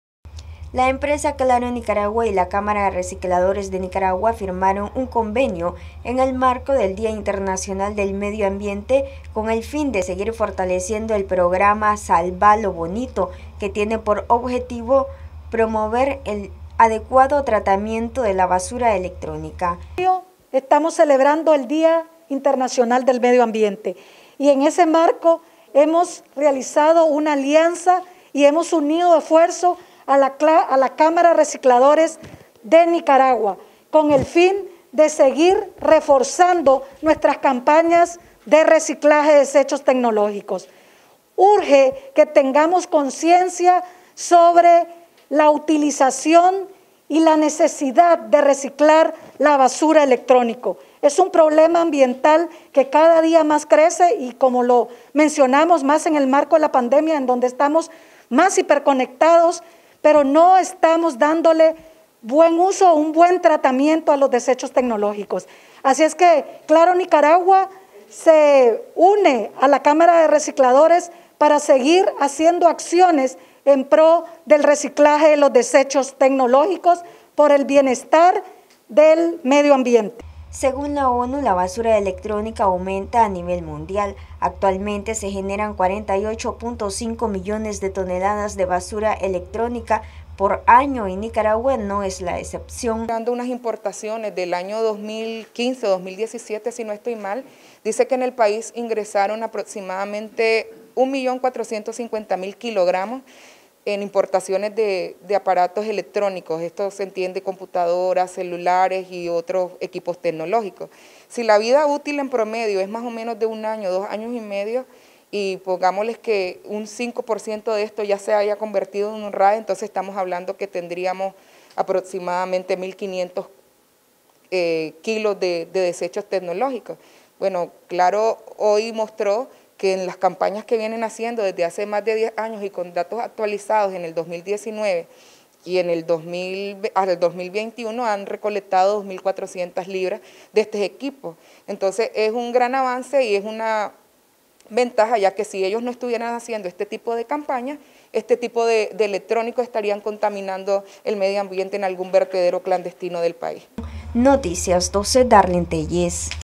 Empresa Claro firma convenio con CARENIC para seguir fortaleciendo el programa ¡Salvà lo bonito!
NosResistimosAlEmbargo Con el fin de seguir fortaleciendo el programa ¡Salvà lo bonito!, Claro firma convenio con CARENIC, para promover el adecuado tratamiento de la basura electrónica